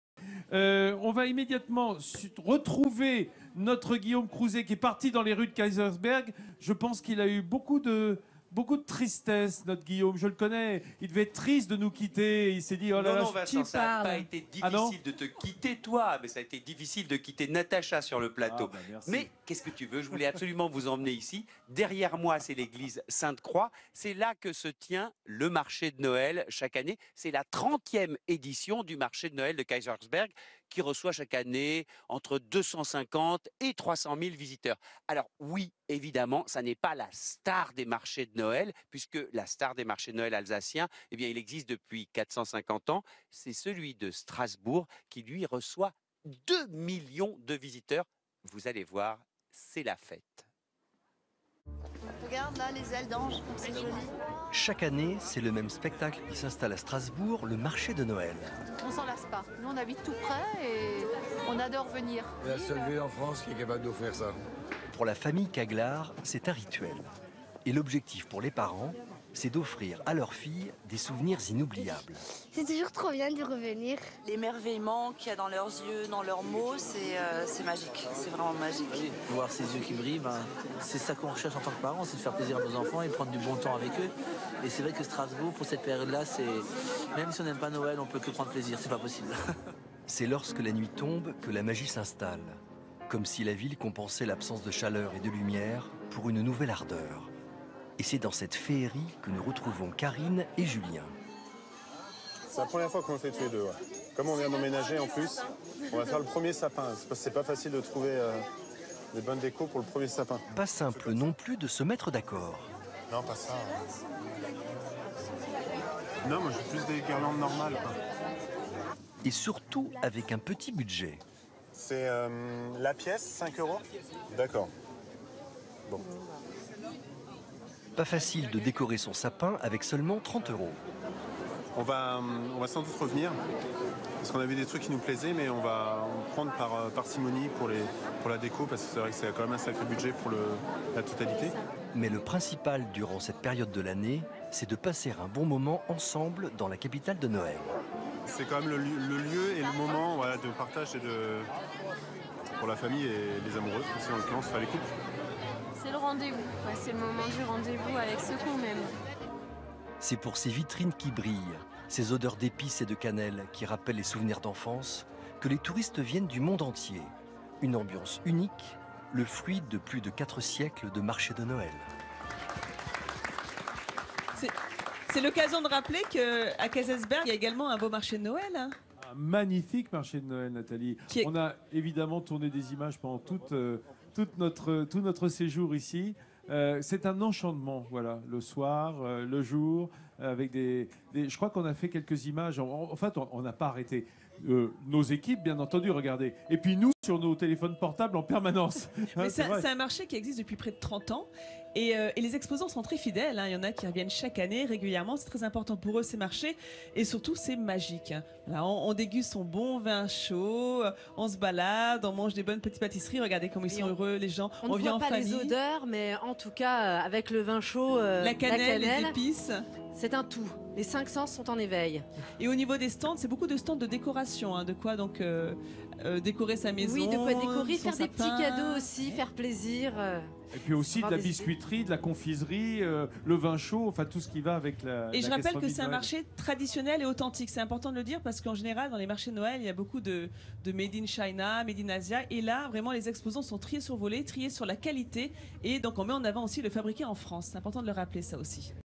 Le Marché de Noël à Strasbourg : un haut lieu du tourisme
Chaque année, c'est le même spectacle qui s'installe à Strasbourg : le marché de Noël. Pour la famille Caglar, c'est un rituel ! Et l'objectif pour les parents,  c'est de créer des souvenirs inoubliables à leurs filles. C'est lorsque la nuit tombe que la magie s'installe, comme si la ville compensait l'absence de chaleur et de lumière pour une nouvelle ardeur.

Infos pratiques :

Le marché de Noel de Strasbourg
Office de Tourisme Strasbourg
Adresse : 17 Place de la Cathédrale, 67000 Strasbourg